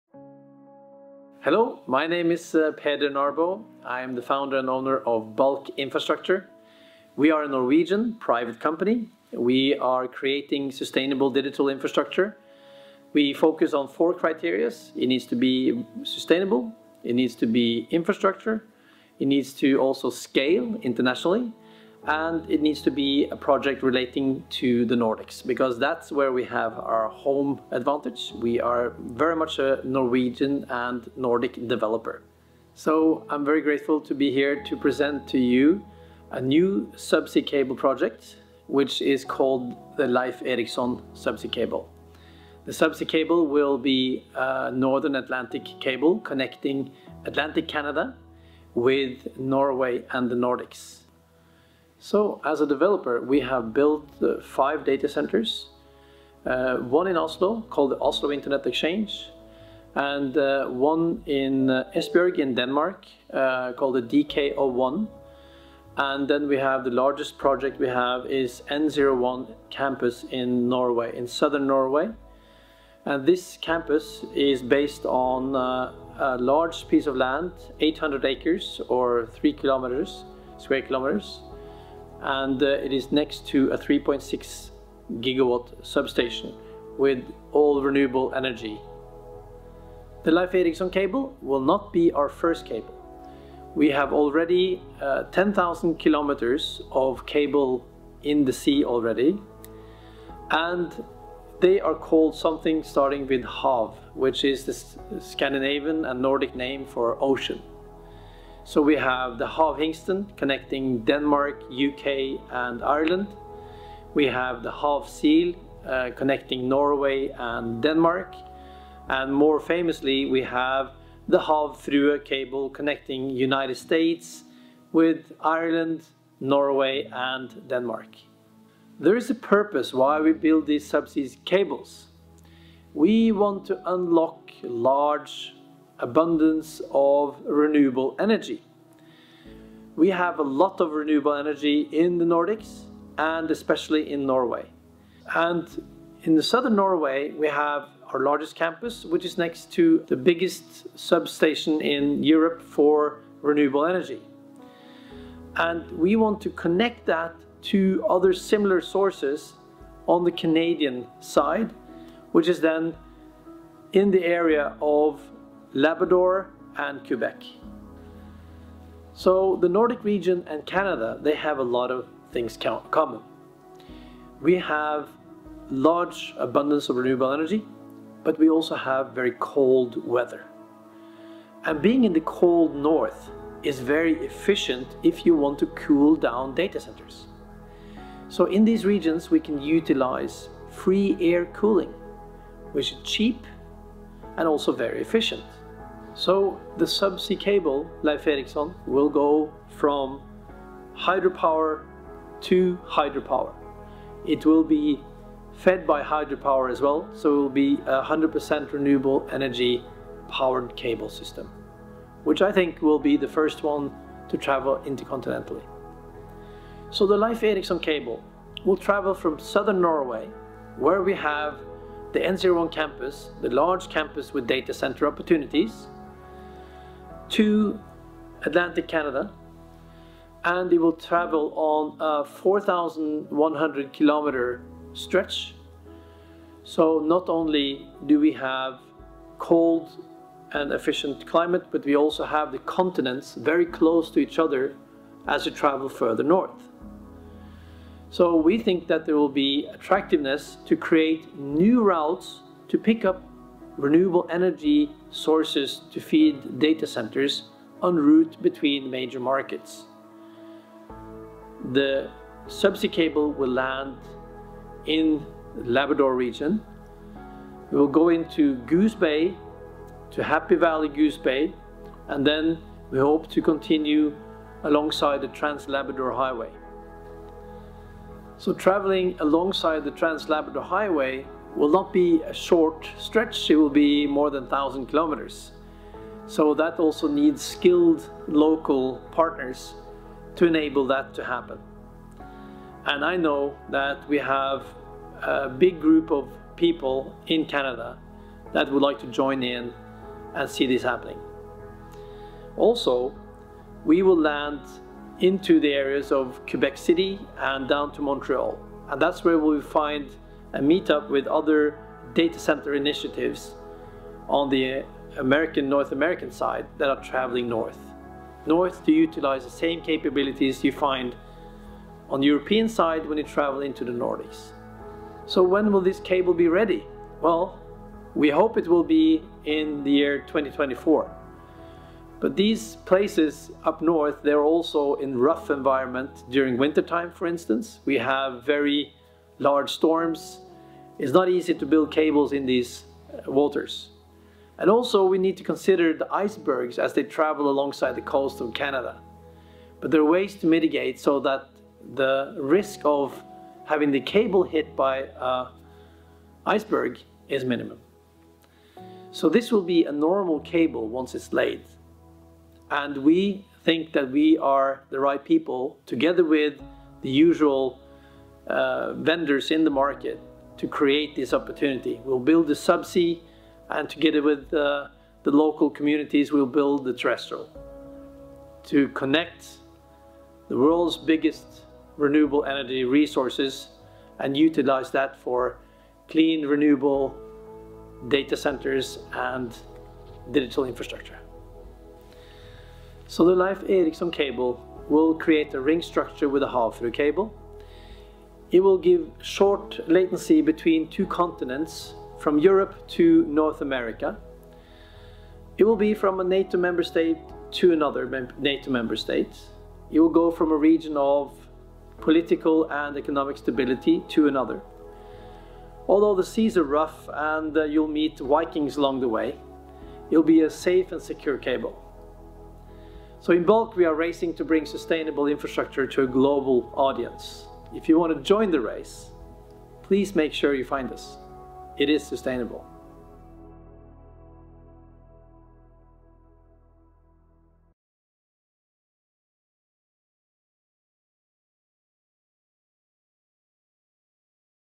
The Leif Erikson Subsea Cable System
Unlocking the World`s Renewable Energy Giants.
The Leif Erikson subsea system will interconnect the two Renewable Energy Giants, the Nordics and Atlantic Canada. We want our digital society to grow in a sustainable way and sometimes solutions to create sustainable digital infrastructure needs new ways of thinking.